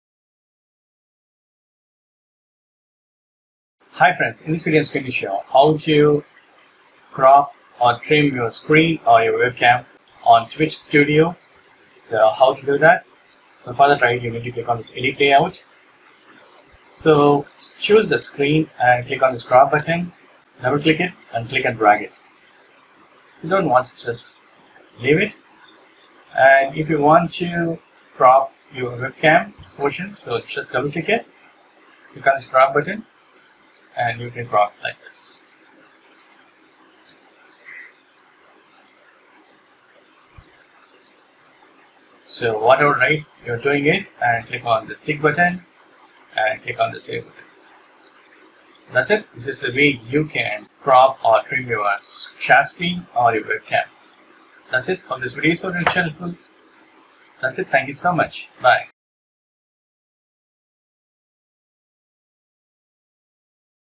Twitch Studio Tips : How to CROP & TRIM Web Cam / Share Screen in Twitch Studio?
hi friends, in this video, i am explaining how to crop and trim web cam or share screen in twitch studio.